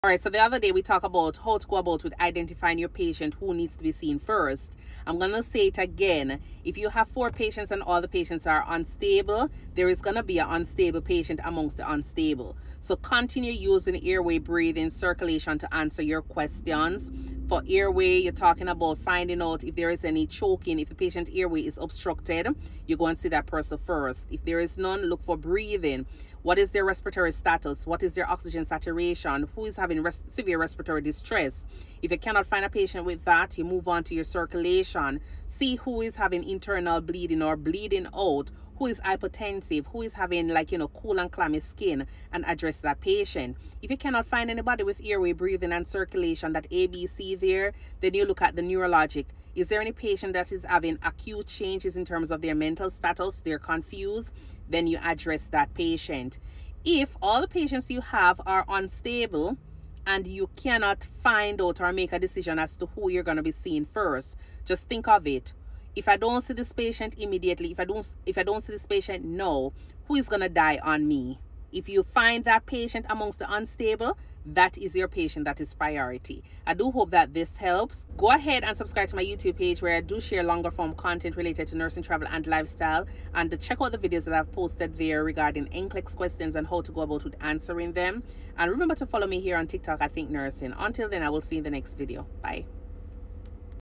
How Nurses Decide Who Needs Help First
Welcome to ThinkNursing! 👩🏾‍⚕️ This video focuses on effective nursing prioritization strategies, especially when managing multiple critically ill patients. We emphasize the importance of using airway assessment, breathing, and circulation principles to guide patient care decisions and identify the most unstable individual. These essential concepts are crucial for nursing school and for success on the NCLEX.

⏱️ Knowing how to prioritize patients is a vital skill for every nurse! In this video, I share strategies to help you decide which patients need care first, manage your workload efficiently, and provide safe, high-quality care.

💉 Topics covered in this video:
 • How to prioritize patients on a busy shift
 • Quick decision-making tips for new nurses and nursing students
 • Common mistakes to avoid in patient prioritization
 • Time management strategies for bedside nurses